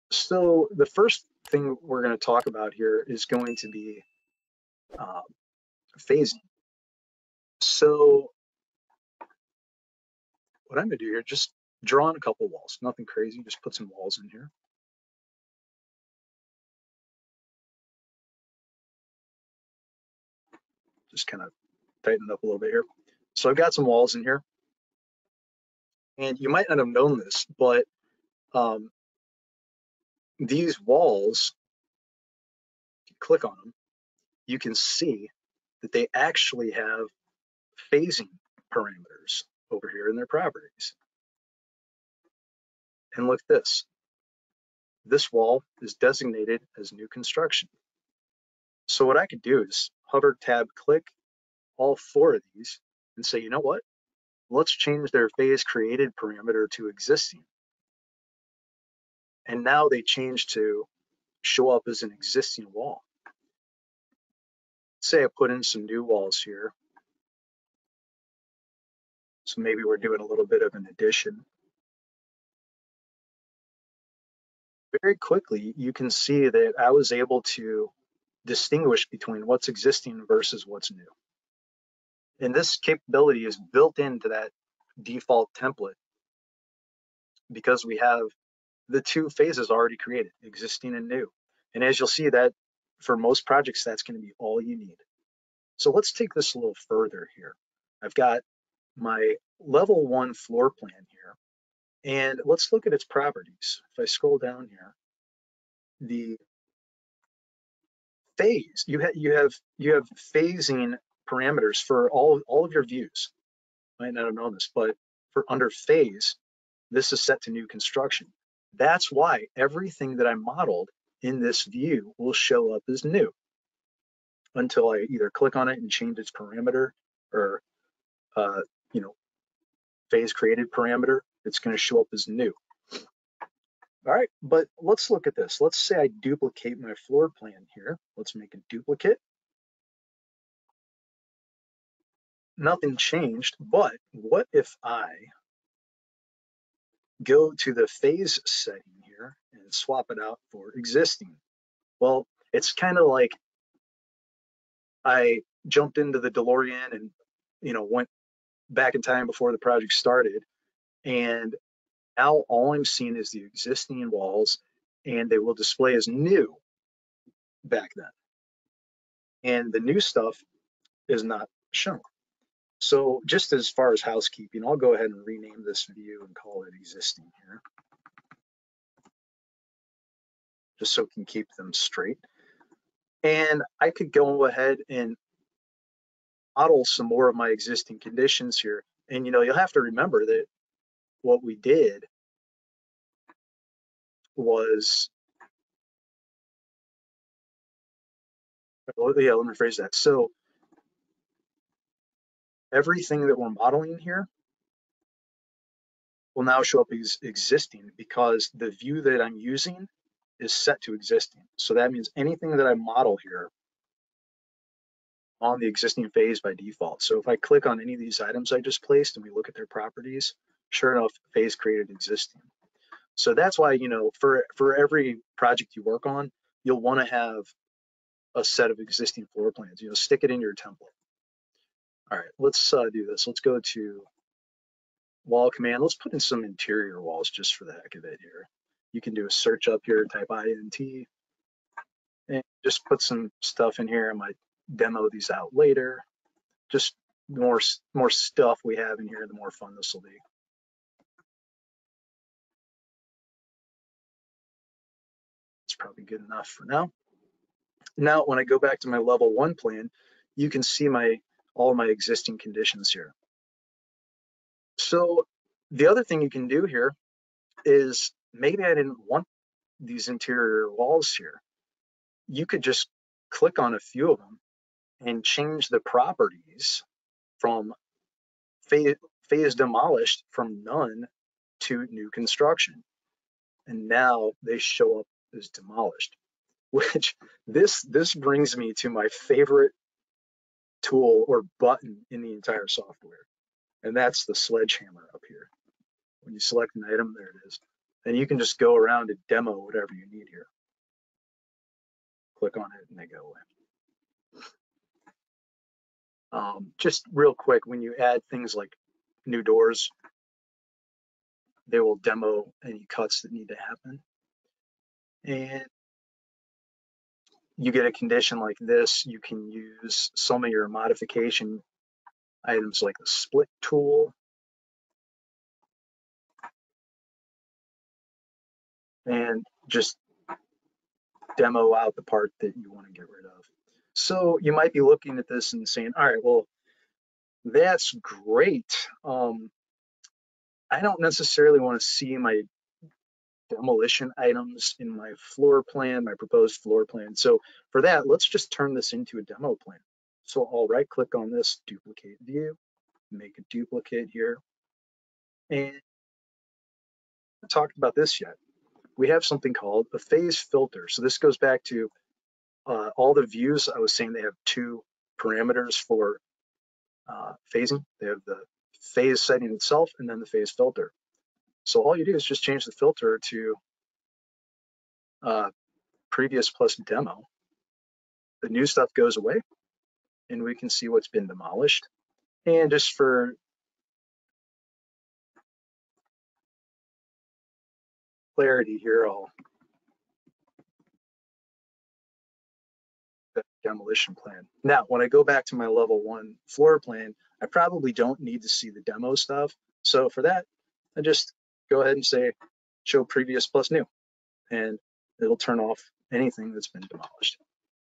Must-Know Phasing Tips in Revit
This is an easy guide for phasing in Revit. We show you how to set up and modify phasing parameters for walls, create and duplicate floor plans for different phases, and use phase filters to clearly distinguish between existing structures, new additions, and demolished elements. Perfect for architects, engineers, and contractors looking to enhance project coordination and collaboration.